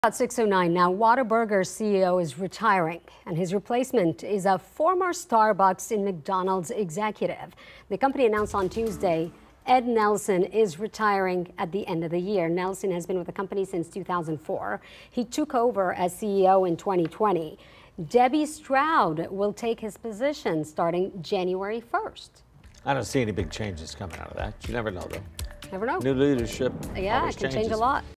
Whataburger CEO to retire; replacement is a former Starbucks and McDonald's exec
Ed Nelson is retiring at the end of the year.